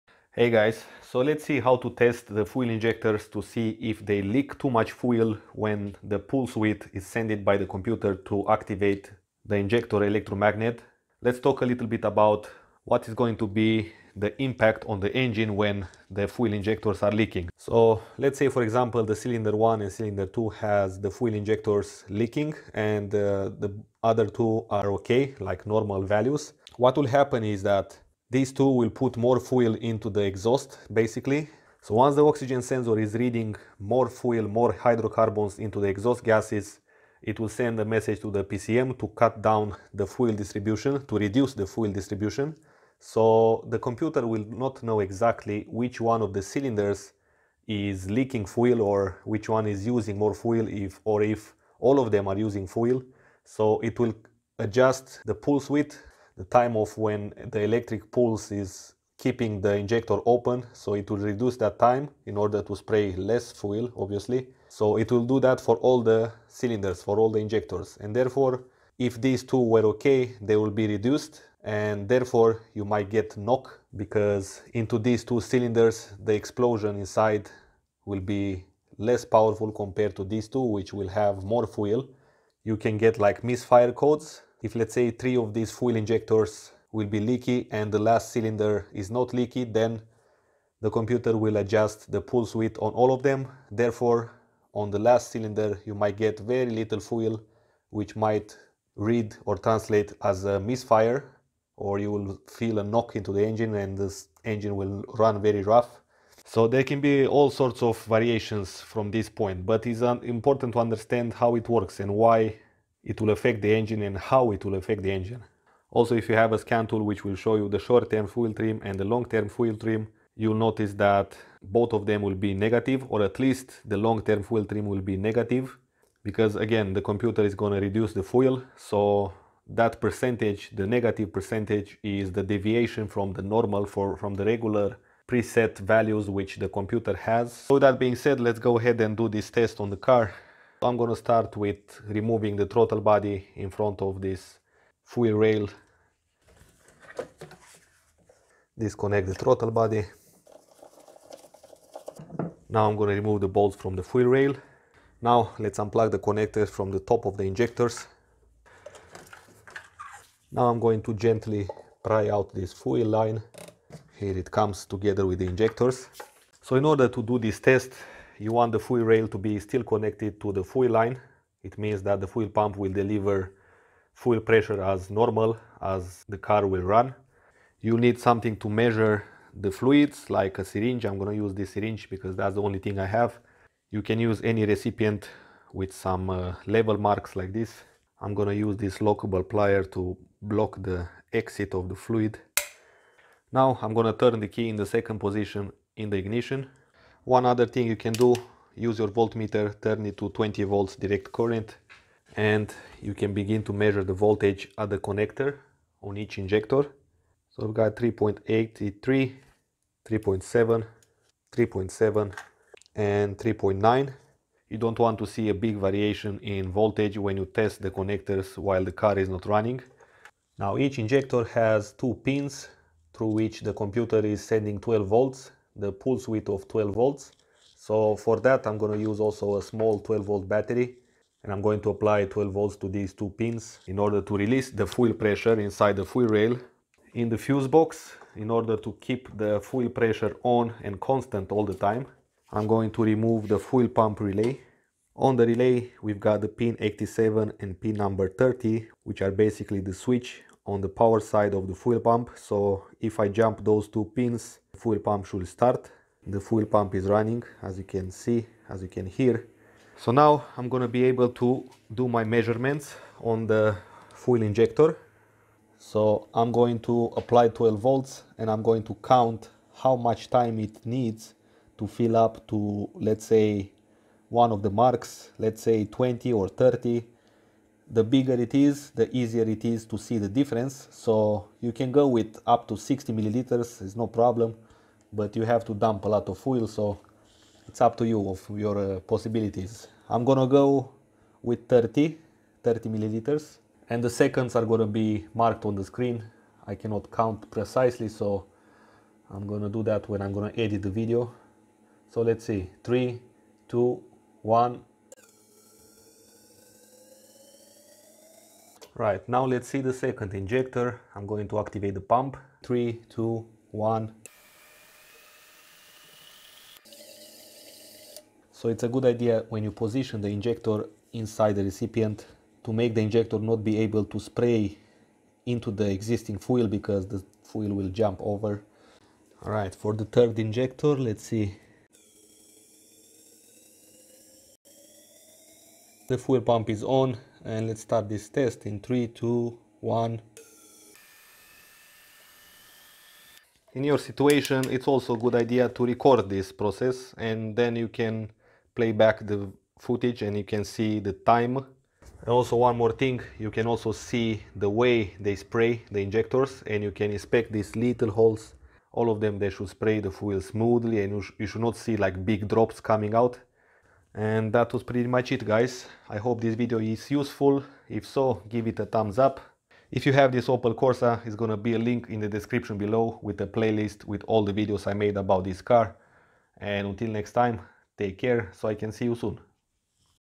How to test the Fuel Injectors for Leaks / Pressure Drop / Multimiter Test DIY
Is importnat to know how to test the fule injectors without expensive equipment and with basic tools . In this video I will show you how to check how much fuel are the injectors delivering into the cylinders and to see if they are leaking .